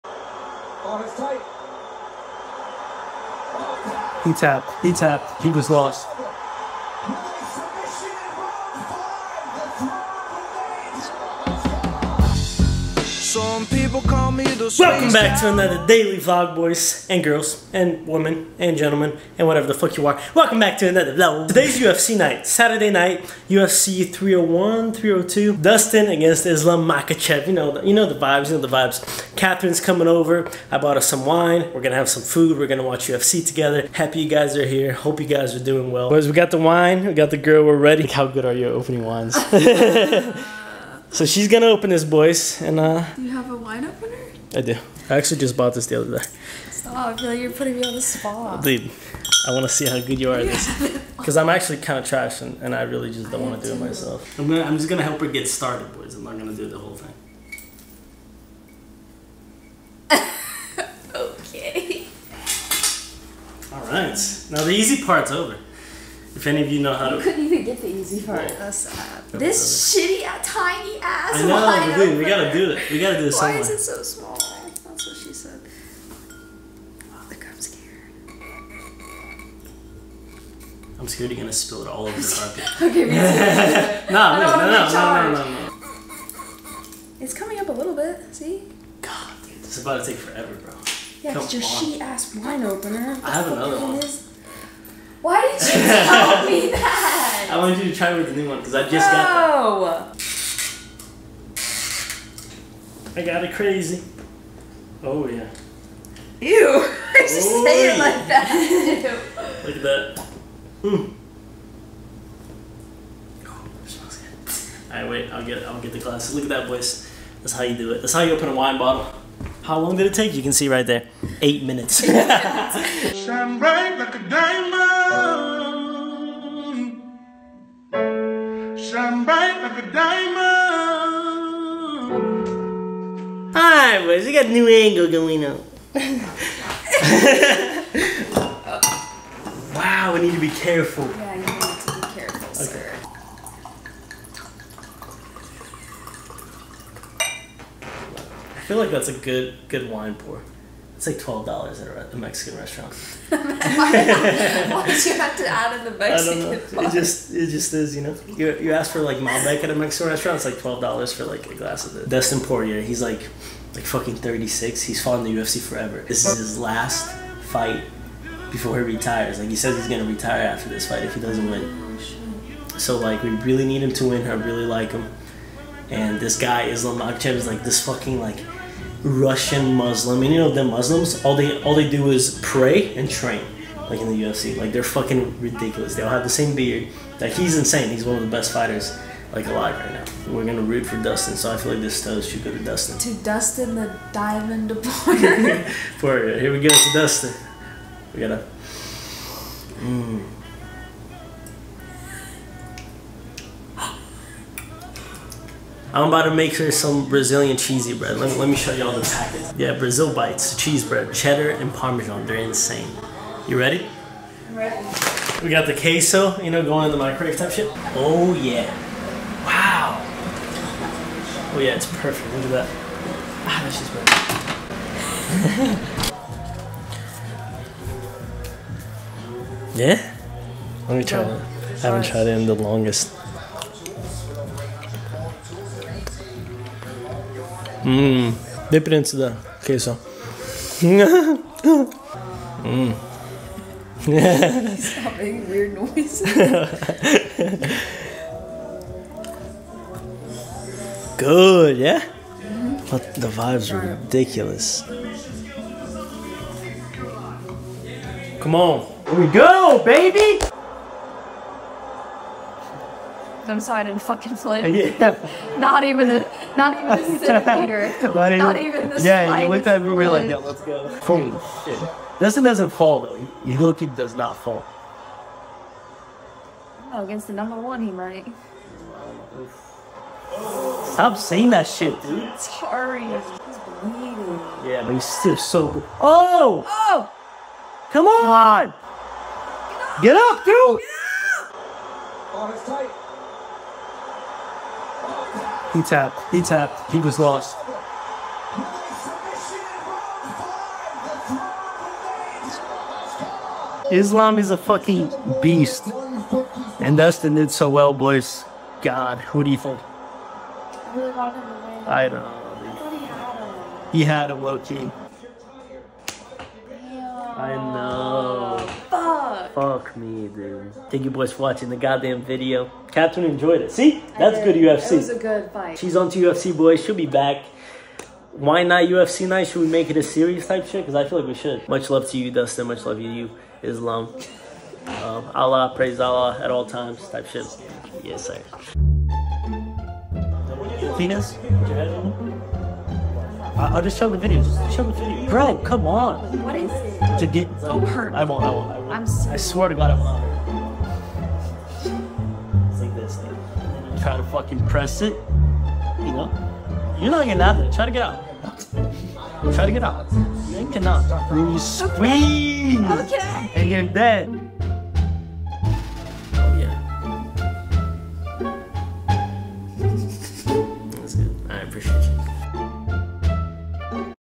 UFC Wine Night With Hot Girl (UFC 302)
Welcome to vlog 238!!! We watch the UFC fights of UFC 302. Islam against Dustin Poirier for the Lightweight Title of the World. We also drink wine.

Timecodes:
00:00 - Intro
00:36 - Wine Opening
02:03 - A Toast
03:25 - Brazilian Bread
04:19 - Islam vs Dustin
05:38 - Outro